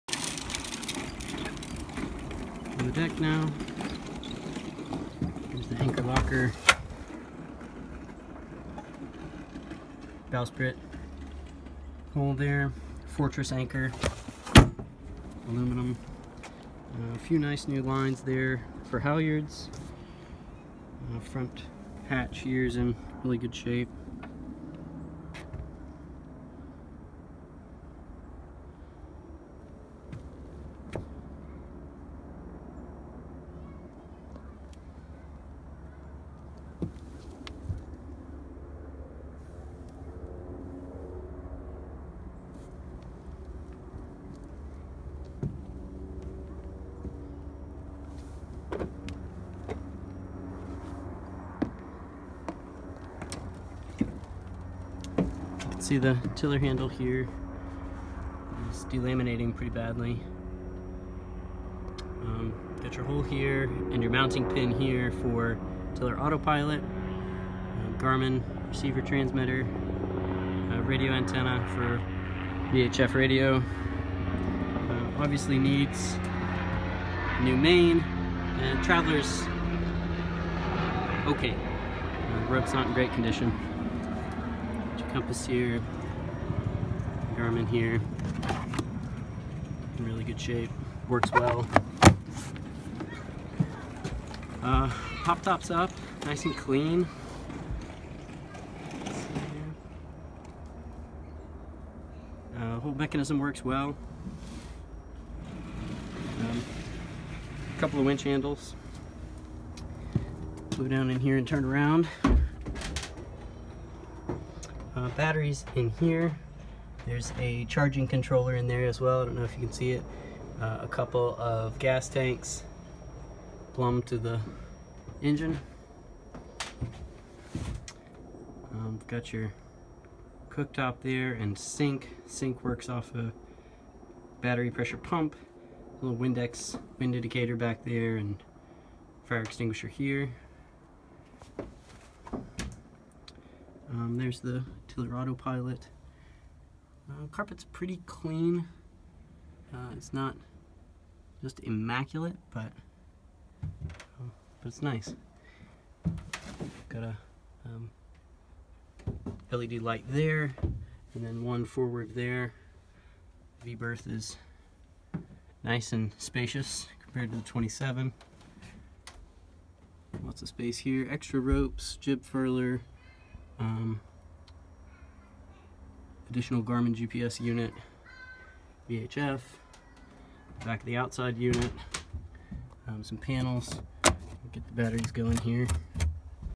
Corsair F28R Trimaran Walk Around Pt 2. - Deck, Cockpit, and Cabin
Curious what an F28R looks like up close? Here's a semi-detailed walk around of the deck, cockpit, and cabin.

I acquired this Corsair F28R at auction. She's in beautiful shape. She's hull number three and has got a racing pedigree that dates back to 1997.

Corsair F28R Trimaran Walk Around - Deck, Cockpit, and Cabin